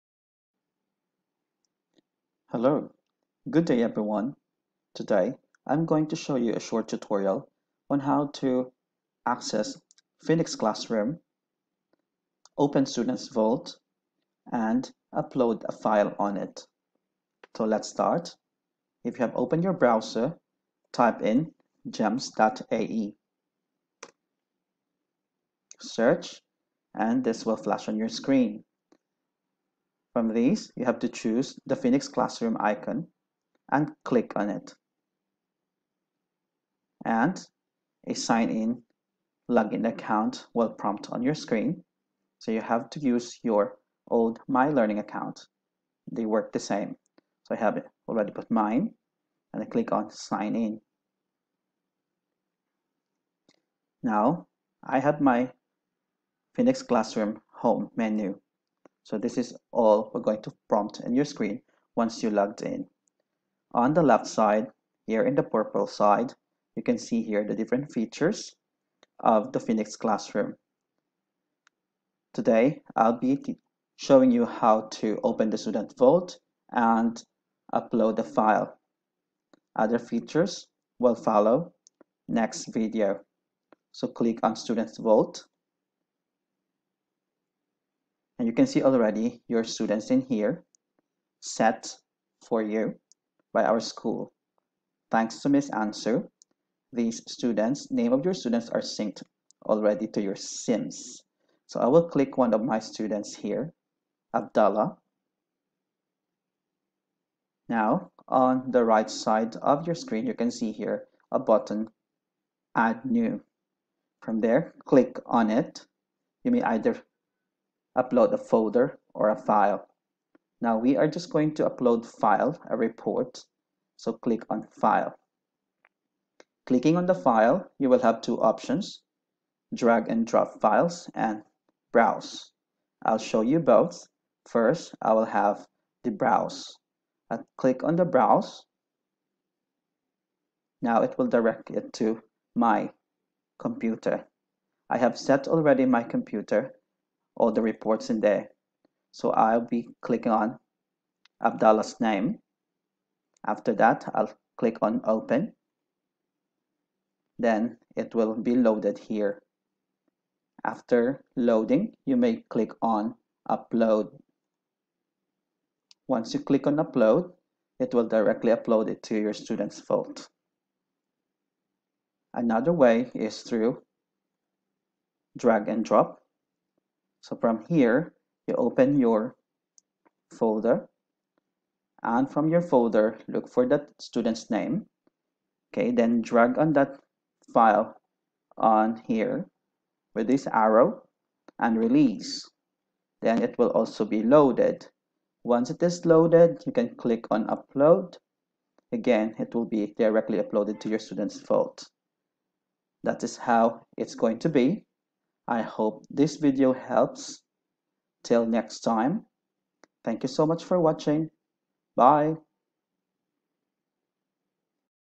For Teachers How to Access Students Vault in Phoenix Classroom
Step by step guide on how to upload students' report on Students Vault via Phoenix Classroom.
WSA